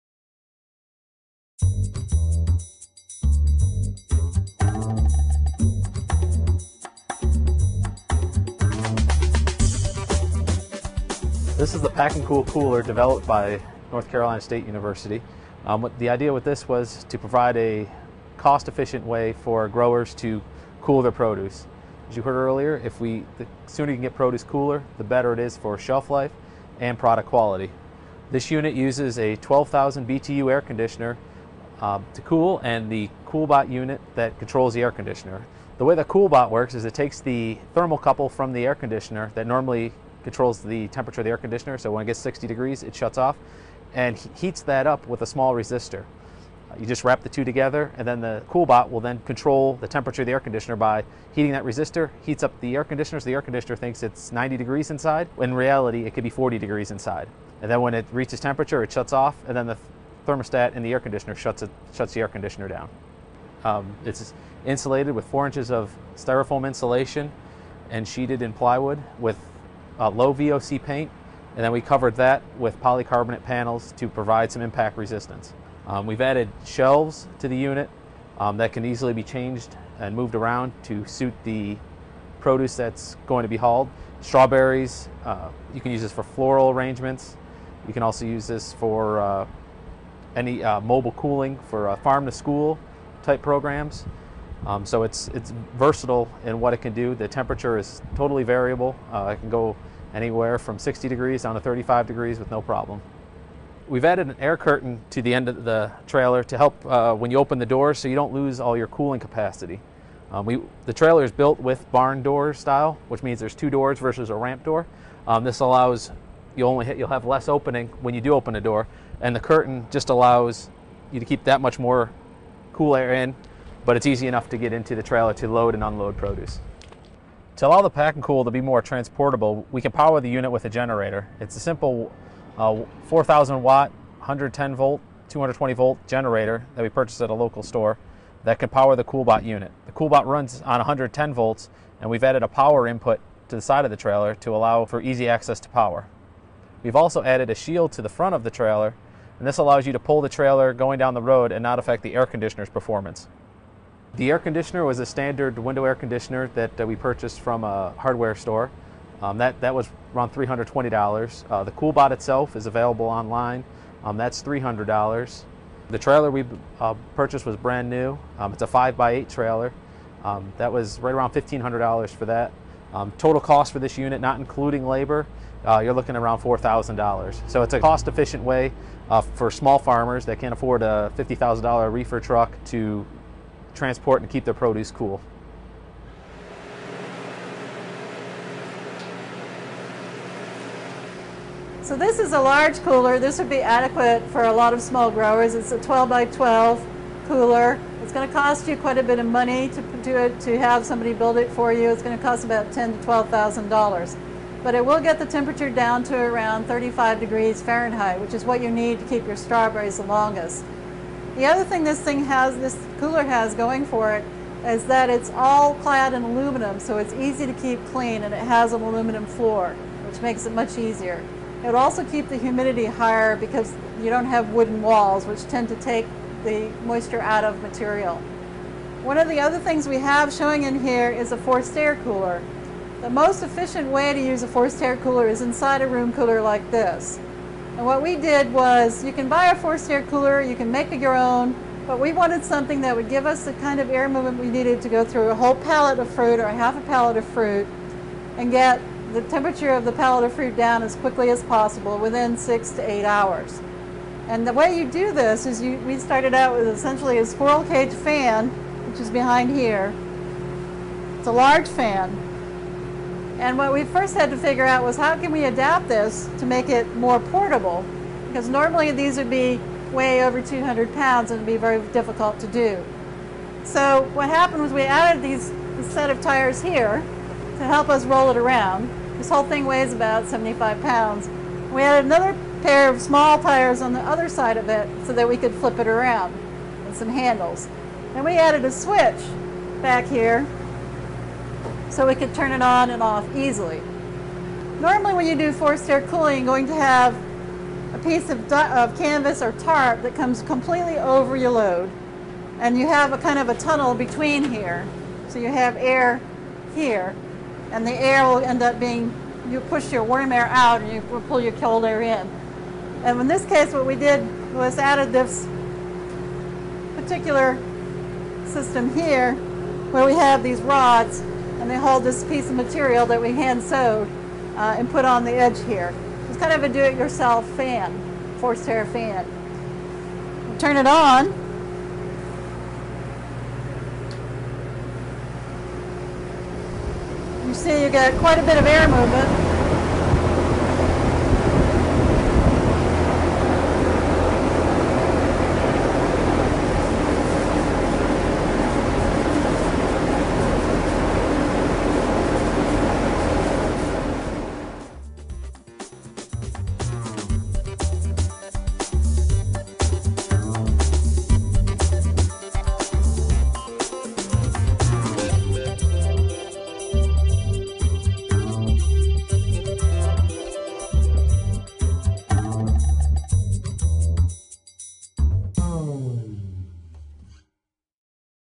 Pack N' Cool and Cold Storage
Louis Wojciechowski of NC State's Plants for Human Health Institute describes the Pack N' Cool trailer developed to cool and transport fresh produce like strawberries. Dr. Penelope Perkins-Veazie then discusses larger coolers and methods to rapidly bring fresh berries down to storage temperature.